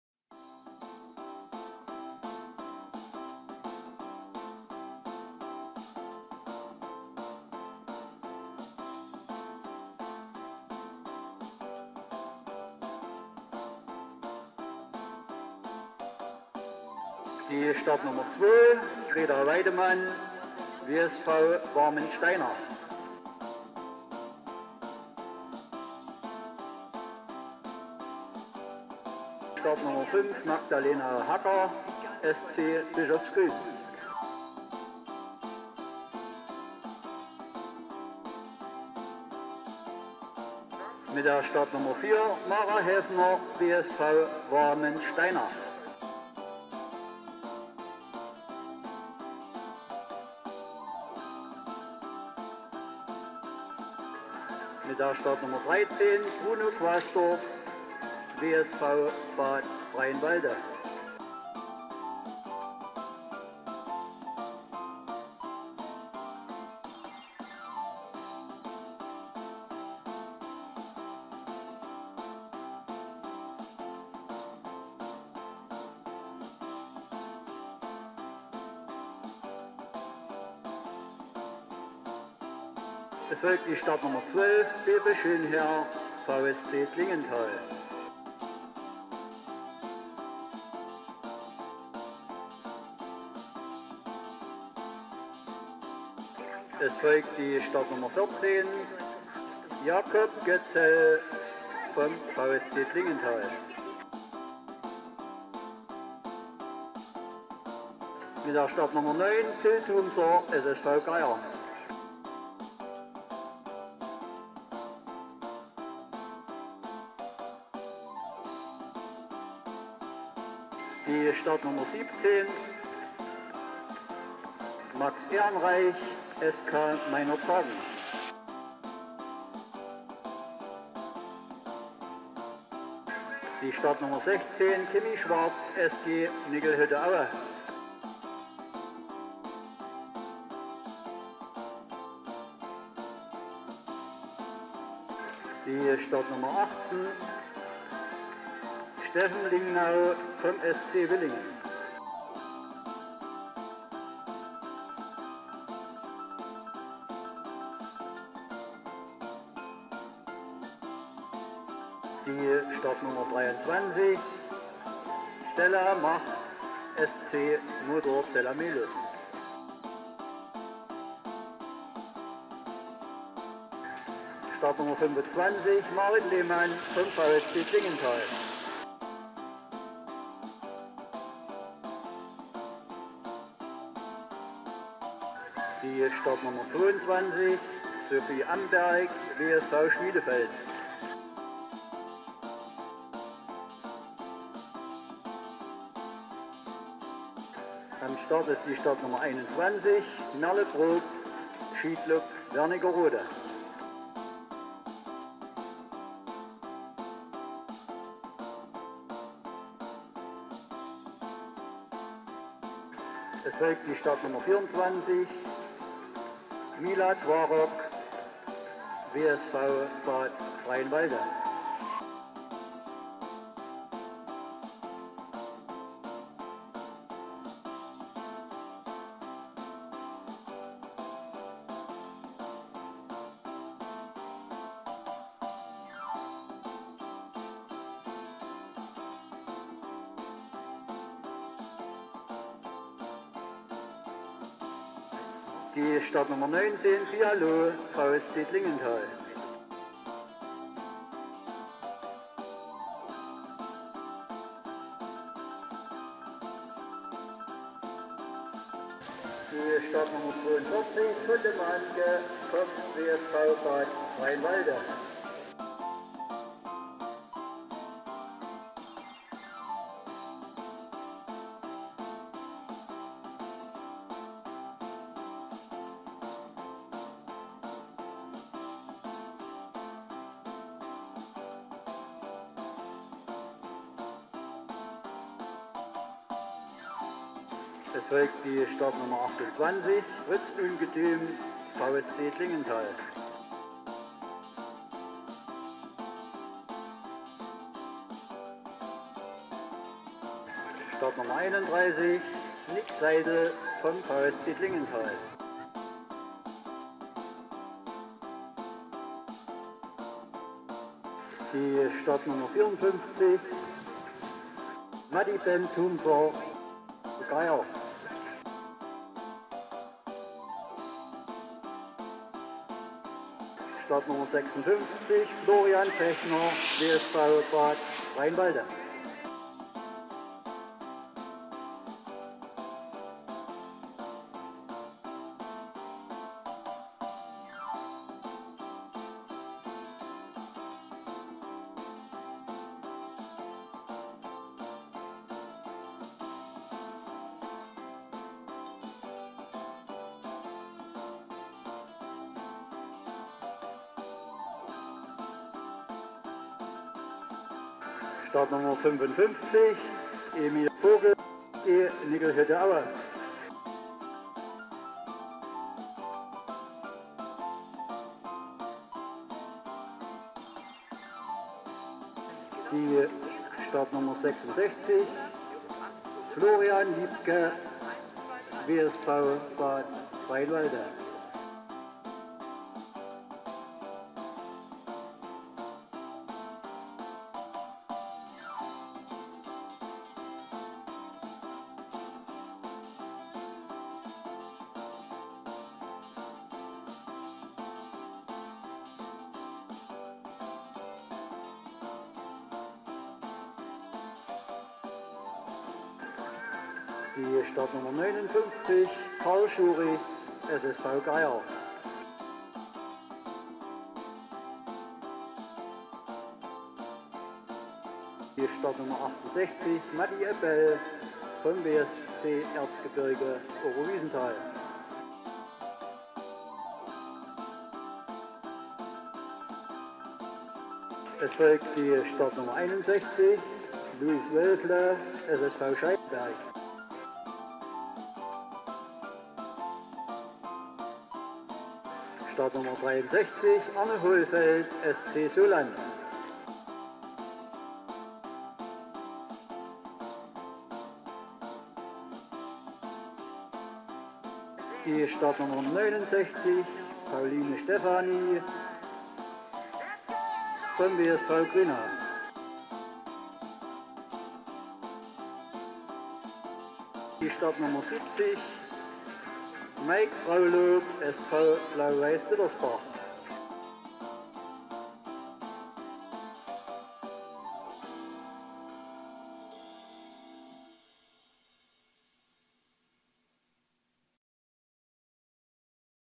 Der NORDCUP in Pöhla
Freude und Unzufriedenheit liegen oft eng beieinander. Hotte Mahnke vom WSV 1923 war am Sonnabend beim Nordcup in Pöhla sichtlich unzufrieden mit sich und seiner Leistung. Aber so ist das nun mal im Sport, manchmal geht eben alles schief. Dann heißt es Kopf hoch, und beim nächsten Mal wieder angreifen, wie die Skispringer sagen.
So fiel dann auch die erste Einschätzung von Landestrainer Stefan Wiedmann geteilt aus. Aber eigentlich hatte er keinen Grund zum Hadern, zwei der fünf gelben Trikots gingen nach dem zweiten Wettkampf der Nordcupserie an den Wintersportverein 1923 Bad Freienwalde! Dabei sind es eigentlich nur vier Gelbe, aber der Kampf bei den 14/15jährigen Jungen ist besonders zwischen dem Bad Freienwalder Florian Fechner und Emil Vogel aus Aue besonders hart. Und so kam es, dass sie in der Cupwertung beide mit der gleichen Punktzahl auf Platz Nummer 1 landeten; zwei gelbe Leibchen waren die Folge. Florian wurde diesmal Zweiter, 1,6 Punkte fehlten ihm zum Sieg. Das zweite gelbe Leibchen holte sich Mila Twarok, die in Pöhla ebenfalls Zweite hinter der überragenden Pia Loh vom VSC Klingenthal wurde. Die ein Jahr ältere Pia startete bei den 14/15jährigen Mädchen zwei Luken tiefer und erreichte trotzdem in beiden Wertungsdurchgängen die größte Weite.
Der Pechvogel des Tages war Bruno Quasdorf, nach dem ersten Durchgang noch auf einem aussichtsreichen vierten Platz, stürzte er im zweiten Durchgang bei immerhin 50,5 Metern und landete schließlich auf Rang 8 unter den 13 Springern in seiner Altersklasse 12/13.
Das insgesamt gute Abschneiden der Adler aus Bad Freienwalde rundete schließlich außerhalb der Nordcupwertung bei den Jugendlichen zwischen 16 und 19 Jahren Florian Lipke mit einem sechsten Rang ab.
Herzlichen Glückwunsch an alle.
In Bad Freienwalde gibt es den nächsten wintersportlichen Höhepunkt bereits am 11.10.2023 um 18.30 Uhr, wenn auch nicht auf den Schanzen. Dann nämlich werden beim Foyergespräch in der Konzerthalle neben dem DSV - Sportdirektor Weltcup Skisprung/Nord. Kombination, Horst Hüttel, auch die Olympiasieger und Weltmeister Helmut Recknagel und Uli Wehling erwartet.